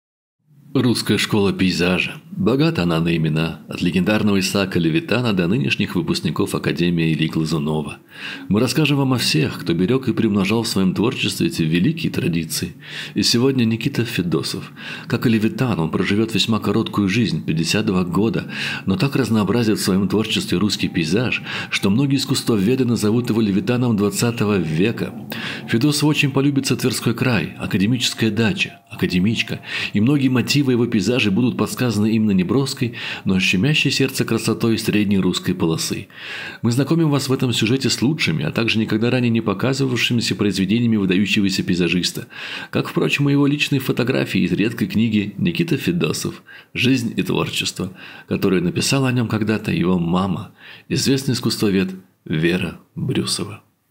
Никита Федосов художник академия Ильи Глазунова Левитан XX века картины искусствовед Вера Брюсова
Русская школа пейзажа…
Богата она на имена. От легендарного Исаака Левитана до нынешних выпускников Академии Ильи Глазунова.
Мы расскажем вам обо всех, кто берег и преумножал в своем творчестве эти великие традиции.
И сегодня – Никита Федосов…
Как и Левитан, он проживет весьма короткую жизнь – 52 года, но так разнообразит в своем творчестве русский пейзаж, что многие искусствоведы назовут его «Левитаном ХХ века».
Федосову очень полюбится Тверской край, Академическая дача, Академичка, и многие мотивы его пейзажей будут подсказаны именно неброской, но щемящей сердце красотой средней русской полосы.
Мы знакомим вас в этом сюжете с лучшими, а также никогда ранее не показывавшимися произведения выдающегося пейзажиста. Как, в прочем, и его личные фотографии из редкой книги «Никита Федосов – Жизнь и творчество», которую написала о нем его мама, известный искусствовед Вера Брюсова.